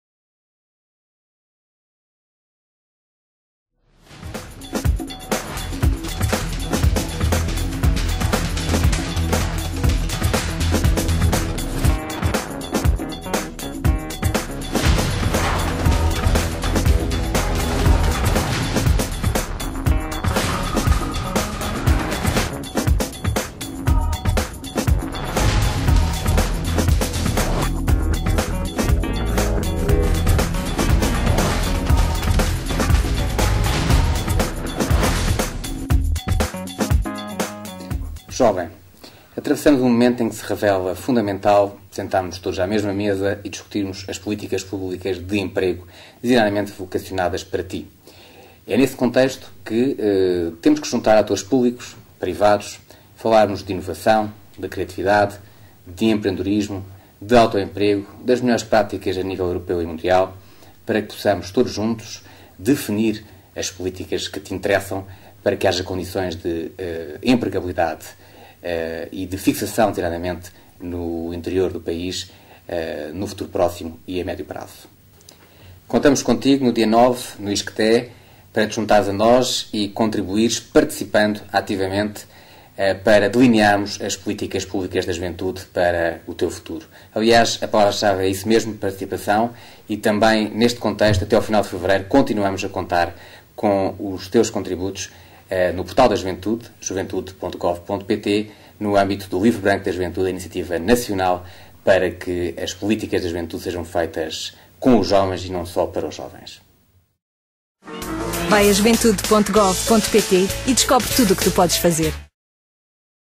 Conferência Emprego Jovem Perspetiva e Horizonte - Lisboa, ISCTE, 09|02|2012
O desemprego afeta atualmente cerca de 30% dos jovens em Portugal.

Com o intuito de equacionar esta problemática, a Secretaria de Estado do Desporto e Juventude (SEDJ) promove a Conferência Emprego Jovem, Perspetiva e Horizonte.

O evento decorre a 9 de fevereiro, no ISCTE-iul, em Lisboa, no auditório/sala B204 e tem como objetivo fundamental:

Refletir, debater e apresentar propostas e soluções para uma dinamização de políticas públicas de promoção do emprego, empreendedorismo e dinâmica económico-social, através de jovens, com um modelo moderno, que vá de encontro dos jovens, interativo e intergeracional.

| Destinatários |
Destina-se sobretudo a jovens entre os 16 e os 30 anos, e também a representantes de organizações empregadoras, representantes de Empresas e outros.

Podes participar no evento de três formas:

1 ) Presencialmente, para tal deves preencher a tua  ficha de inscrição até 7 de fevereiro;

3) Podes também assistir ao evento nas Lojas Ponto JA do IPJ, através de plasmas e colaborar em grupo, ou individualmente, pondo as tuas questões;

| Informações |
Para mais informações sobre o evento, contacta:
Linha da Juventude: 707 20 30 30

Contamos com a tua presença e com a tua colaboração via Internet!!